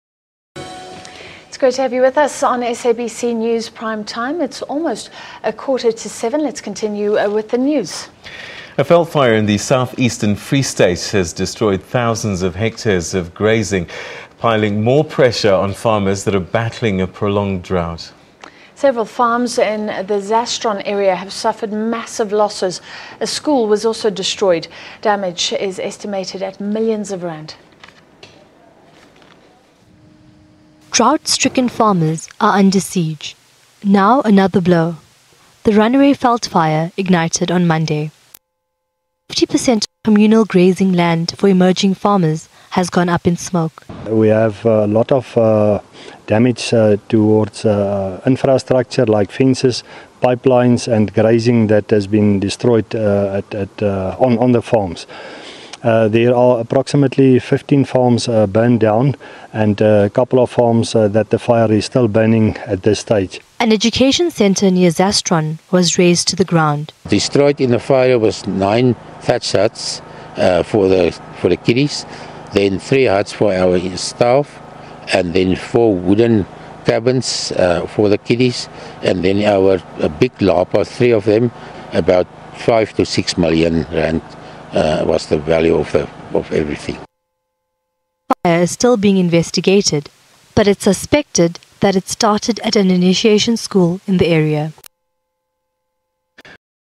South-Eastern Free State veldfire destroys hectares of grazing land
A veldfire in the South-Eastern Free State has destroyed thousands of hectares of grazing. Piling more pressure on farmers battling a prolonged drought.